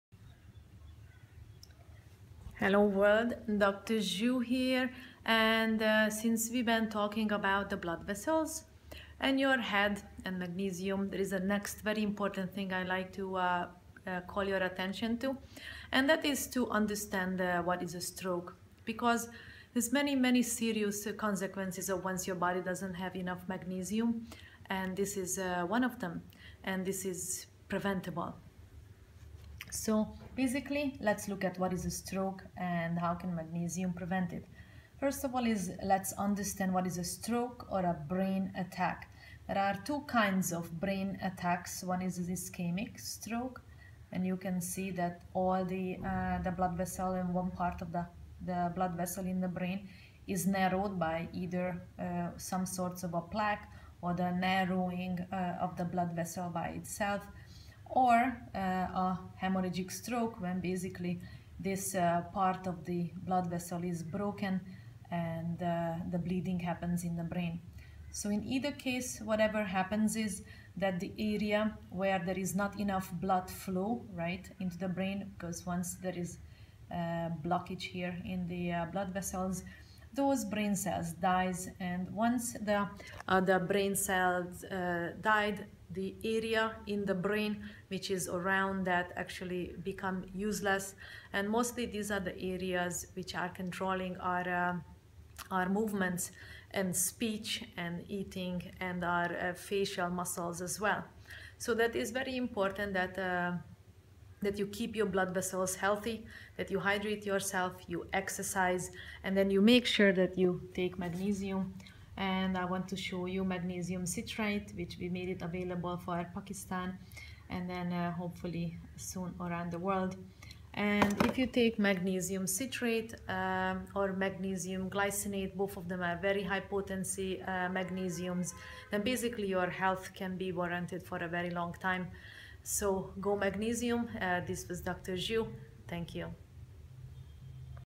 What is a stroke and how magnesium can help to prevent it
Magnesium is used in 325 places in the body and highly likely that that you have a health issue which is magnesium deficiency related.
Dr Zsu shares what is a stroke and how can one prevent it by healthy magnesium levels.

Ask questions from dr Zsu